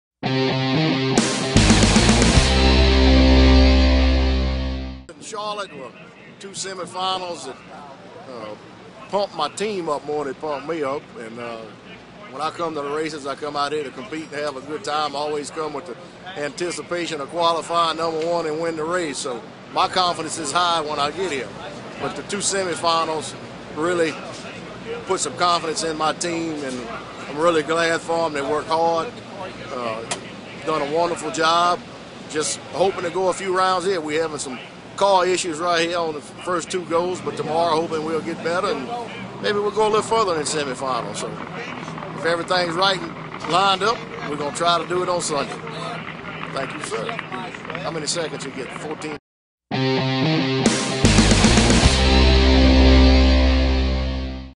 VIDEO NOTEBOOK: KENT REVELS IN SUCCESS AND HAVING FUN
Steve Kent is a serious drag racer and for him, the part he's most serious about is in having fun. The freshman drag racer discusses his recent semi-final finishes in Charlotte and Dallas.